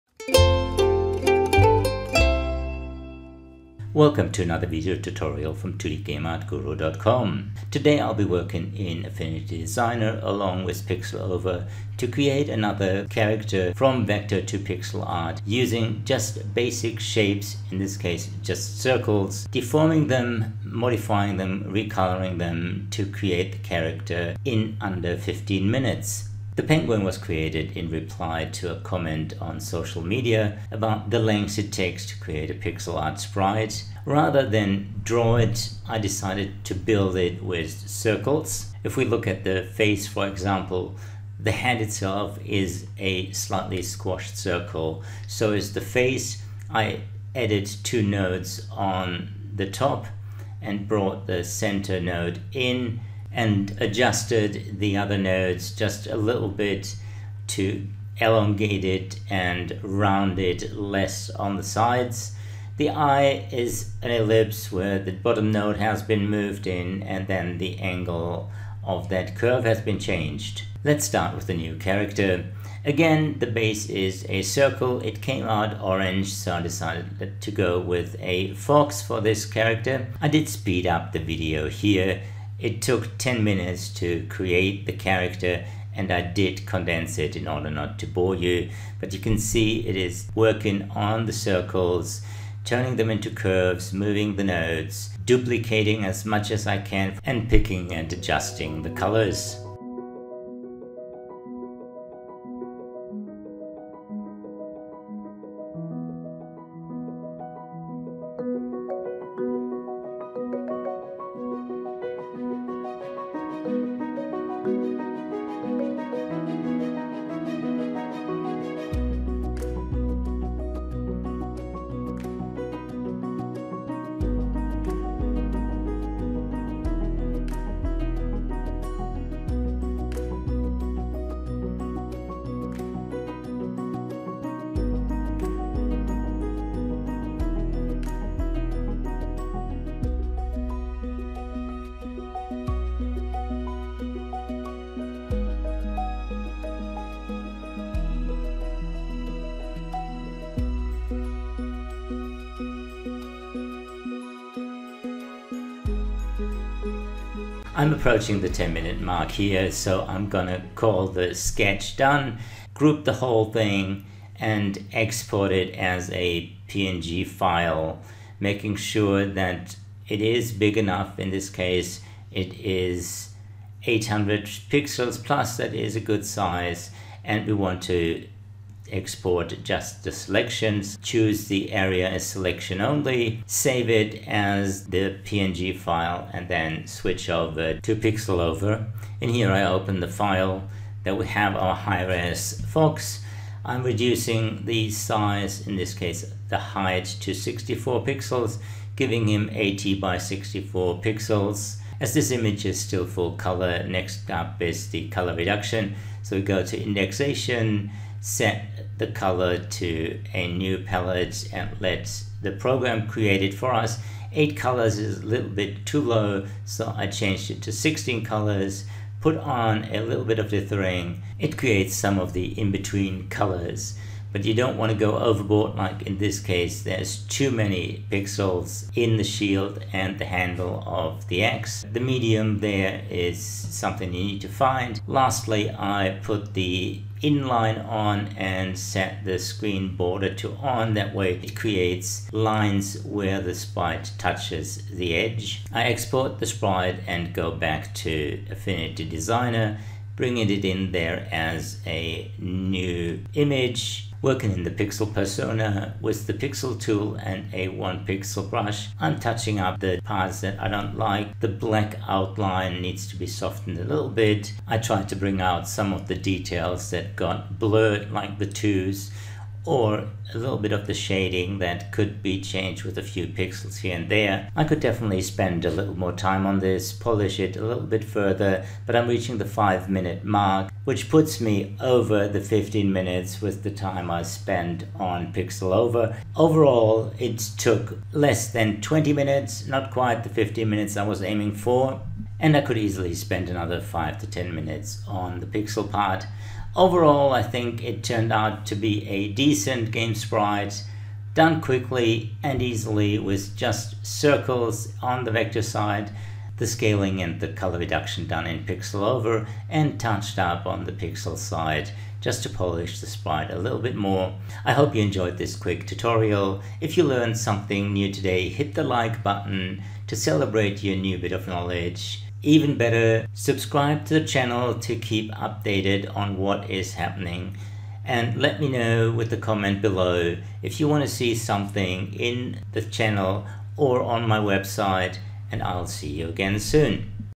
Affinity Designer Tutorial - Game Art & Character creation - from Vector to Pixel with just circles
This is a quick video tutorial from 2dgameartguru.com on game character creation focusing on speed and ease of creation. Instead of drawing the character, I build it in vectors using only circles - trying to finish the game art in 15 minutes.

This video is using Affinity Designer and PixelOver to create game characters quickly and easily a pixel game character. The base shapes are all vector circles, converted to pixels with the help of PixelOver, and touched up a little after.

Most of it can be created with other software tools such as Adobe Illustrator, CorelDraw, or Inkscape with minor alterations and different names for the tools used.

See more Affinity Designer and Inkscape tutorials on the